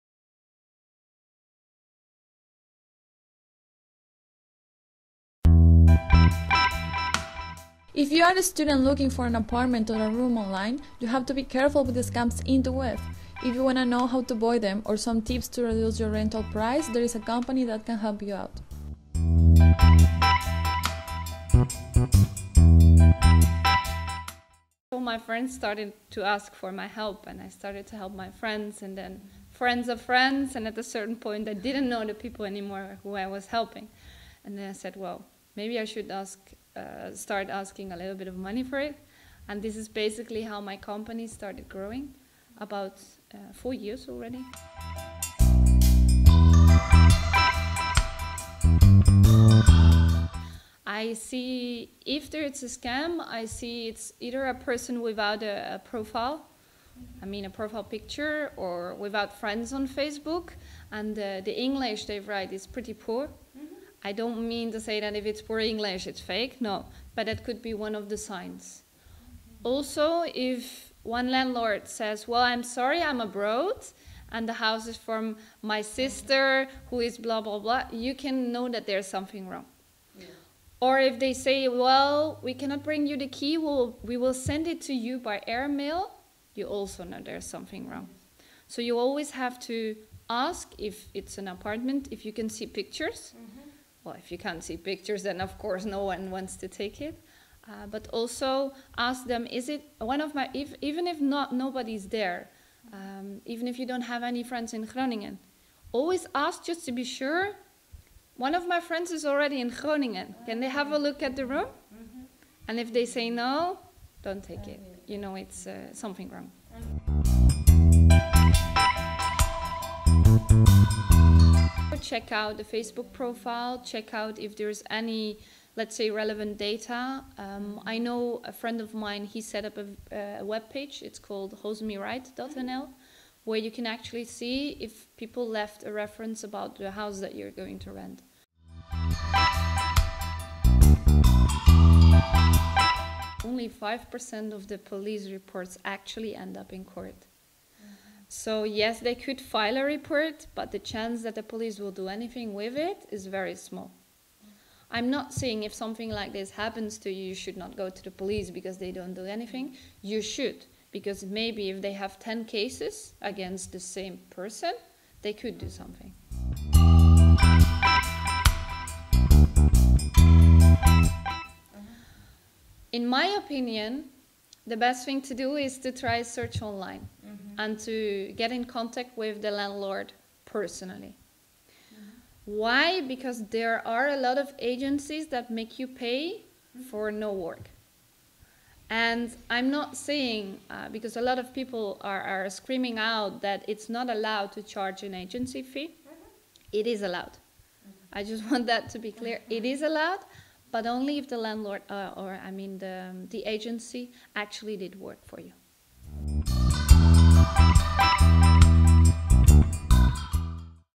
Housing scams in Groningen: How do you prevent becoming a victim?
Scammers are taking advantage of international students looking for rooms, and they are getting away with it. Denise Zonnebeld gives advice on how to spot a scammer and avoid losing lots of money.